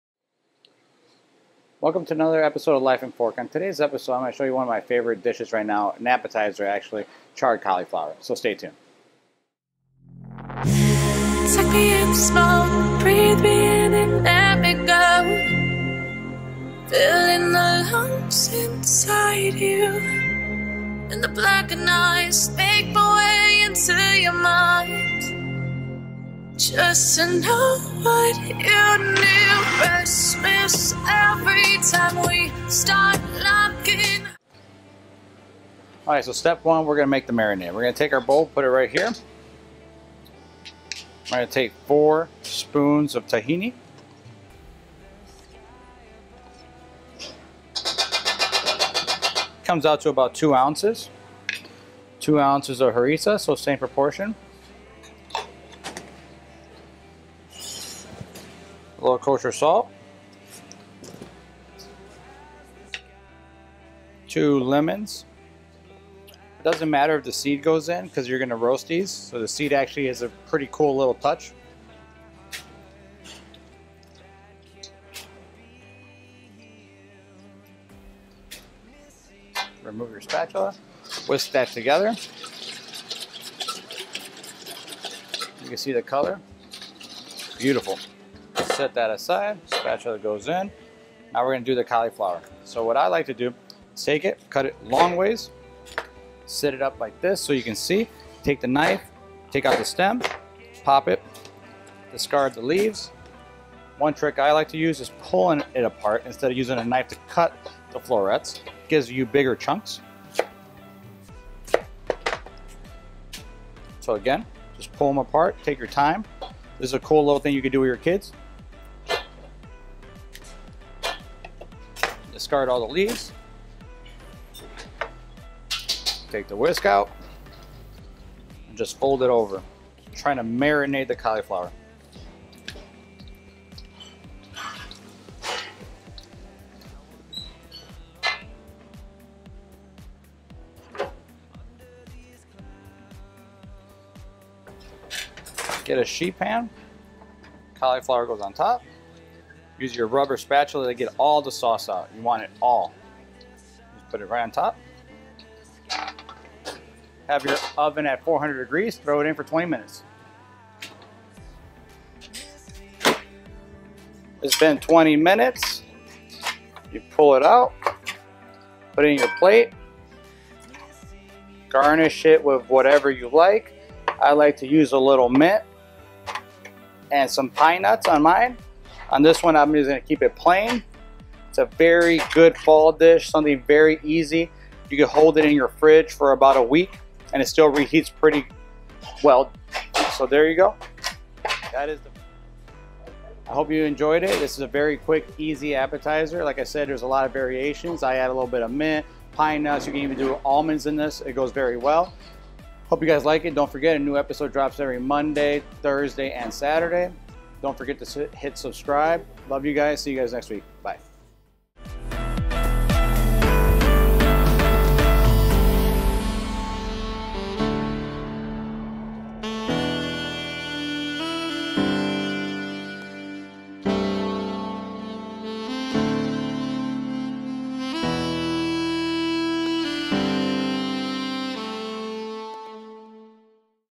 Charred Cauliflower Steak: Appetizer
Charred cauliflower is everywhere right now, first its a great side dish and second its vegan. So let me show you my way of making this famous side dish. Love you guys. New video drops every monday thursday and saturday.